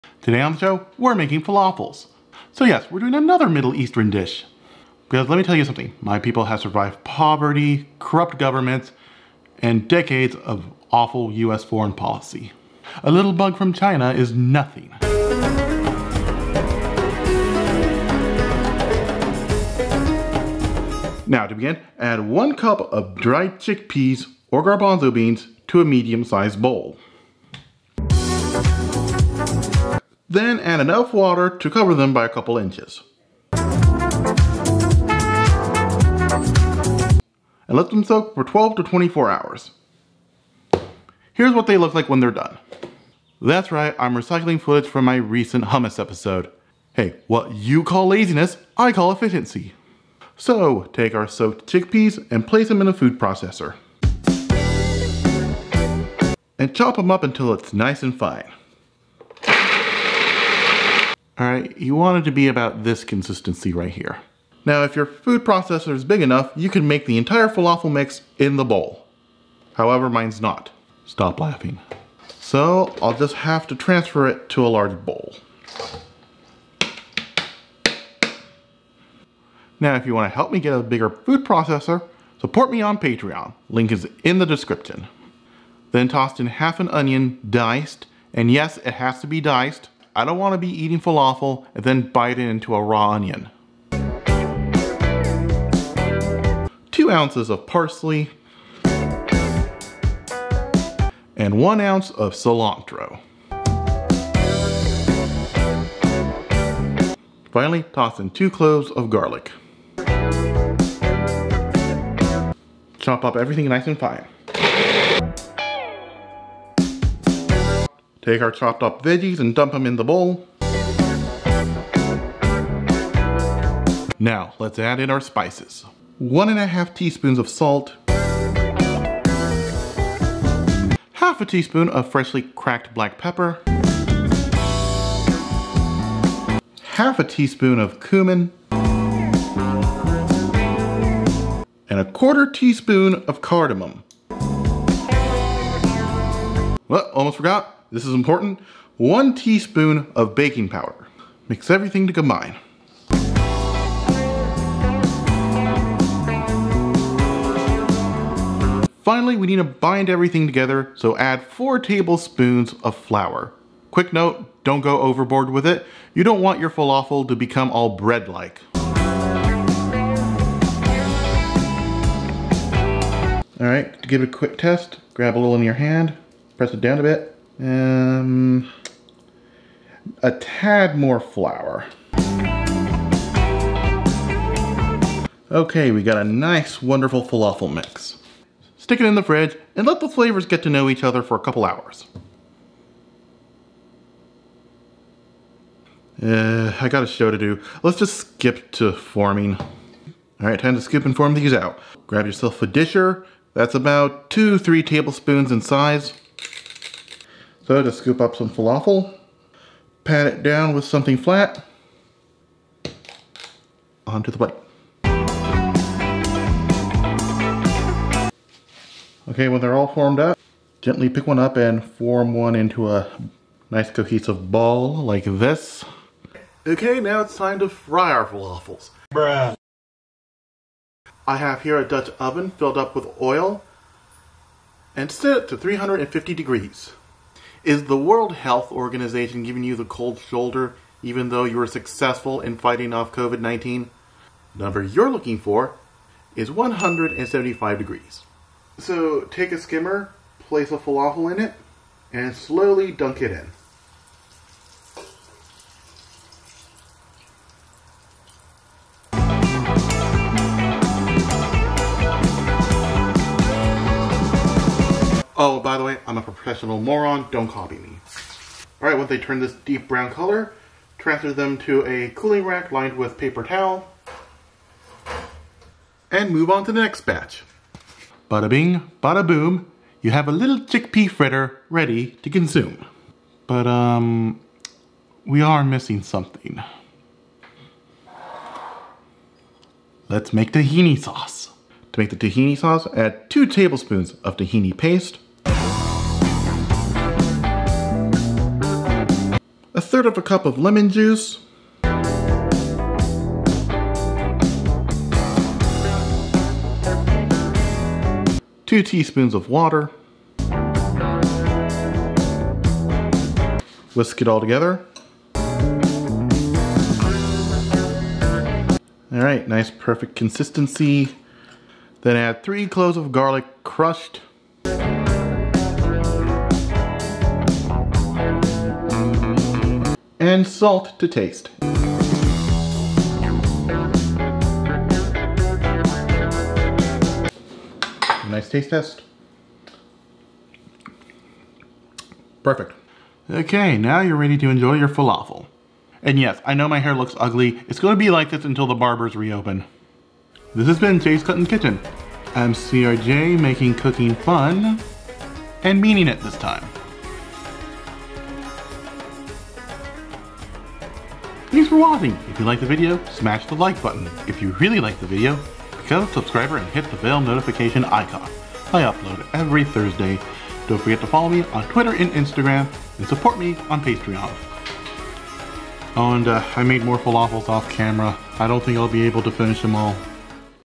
Homeamde Falafel Recipe and Tahini Sauce
In yet another Middle Eastern recipe, I show you how to make the garbanzo bean fritter:  Falafel.  Huh, I just realized Arabs make awesome vegan food.  Much better than any woke Brooklyn hipster dare I say.